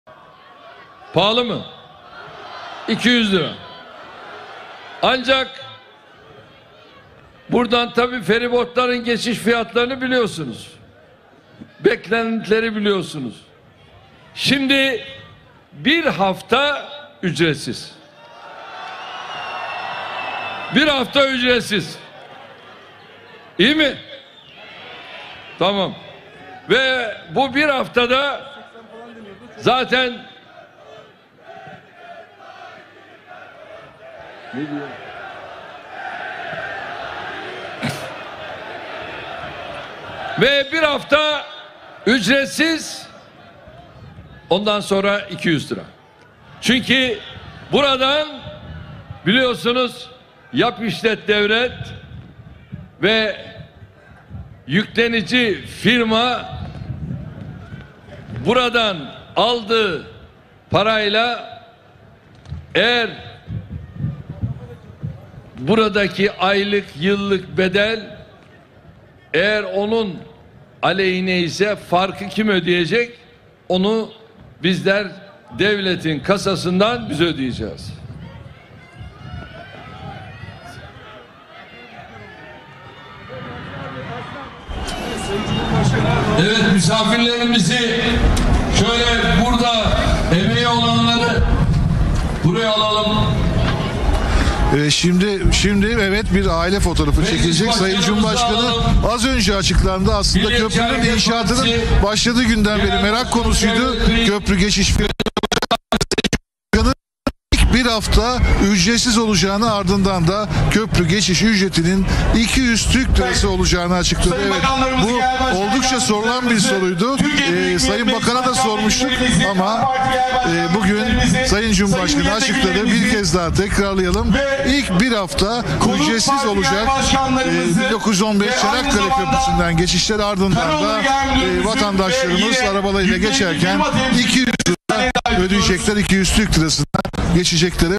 1915 Çanakkale Köprüsü'nün Geçiş Fiyatı Belli Oldu - Cumhurbaşkanı Erdoğan Açıkladı
Cumhurbaşkanı Erdoğan 1915 Çanakkale Köprüsü'nün açılış töreninde konuştu. Yapılan törende köprünün geçiş fiyatını açıklayan Cumhurbaşkanı Erdoğan köprülerden geçişin ilk hafta ücretsiz ardından köprüden geçiş bedelinin 200 lira olacağını söyledi.

TÜRKSAT 4A
Frekans: 12729
Sembol: 30000
Polarizasyon: V (Dikey)